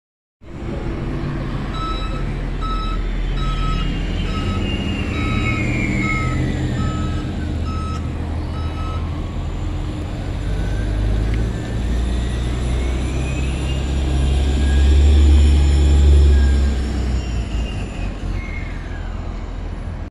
SF Muni 2007 Orion VII HEV 07.503 8526 on Route 56 Rutland
Recorded on September 20, 2022 at San Bruno & Arleta.

Engine: Cummins ISB
Hybrid System: BAE Systems HybriDrive
Division: Woods
Route: 56 Rutland
Direction: Inbound to Visitacion Valley